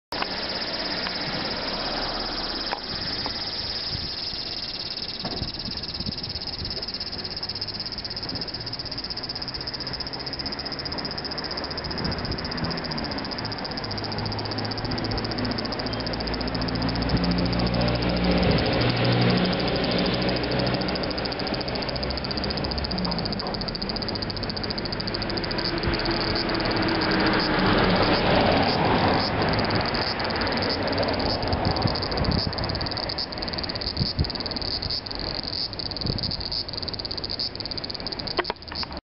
Japanese cicada
The chirping of cicada's in Japan. I forgot I shouldn't tilt the camera but you can still hear the sound the make.